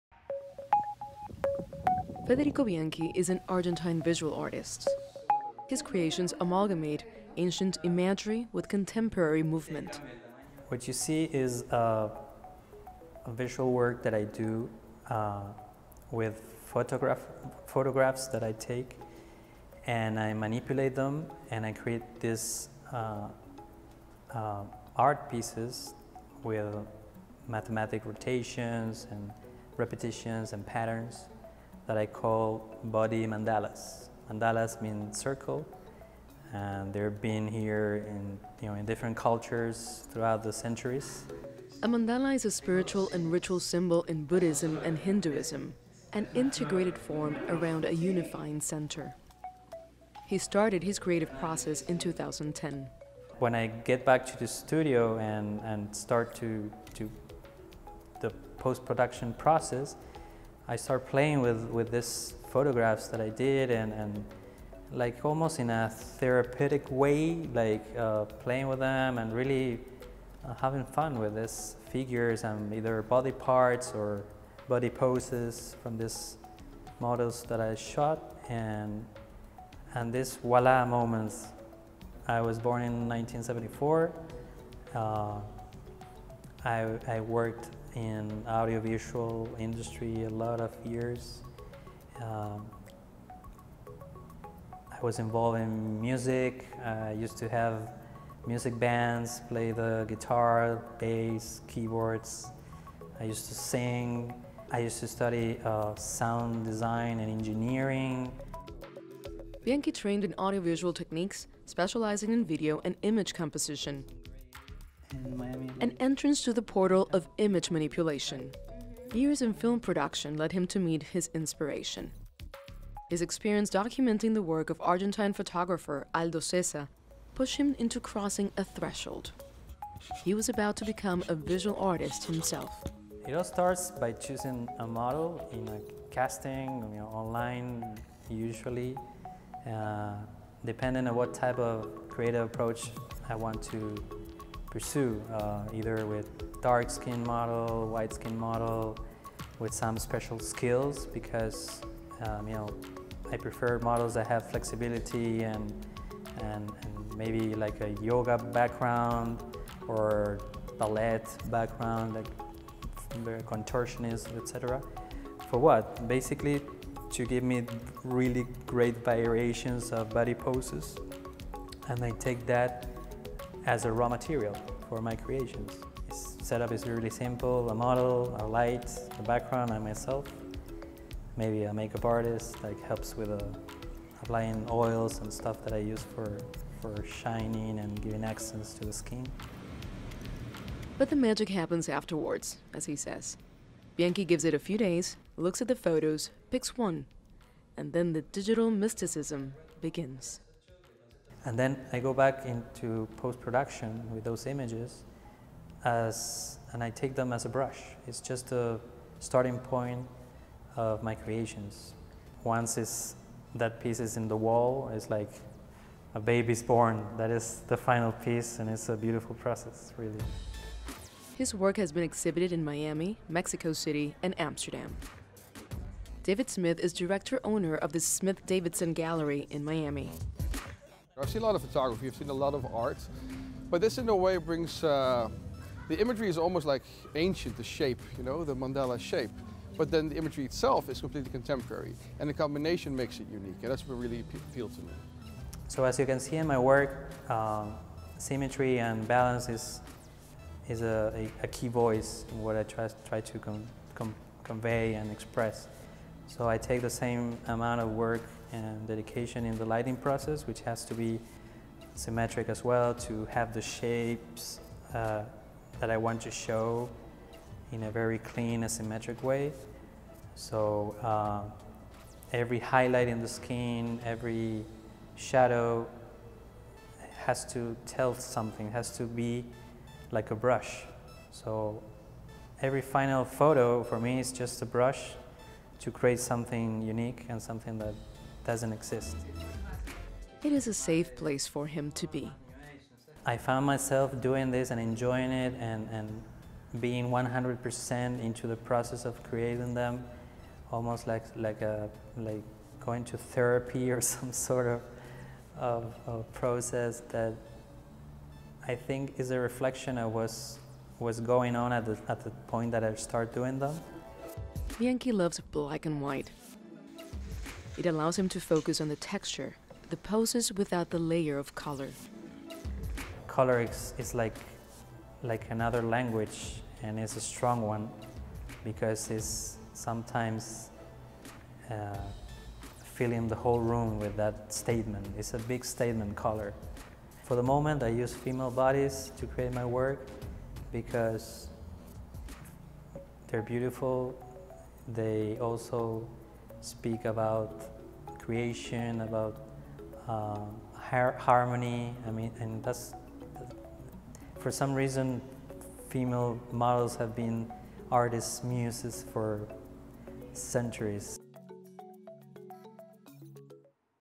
Federico Bianchi interview in CGTN TV Network
With his “body mandalas” project, Federico uses the matrix of the female body to build intriguing and complex images, capturing the dance of light and movement in those archetypal geometries, which summarize the image of the universe and a mystical sense of oneness.

This vision and artistic representation of Federico’s inner state, is complemented by the viewer’s own interpretation of each “body mandala”, and concludes that the game of creation is limitless.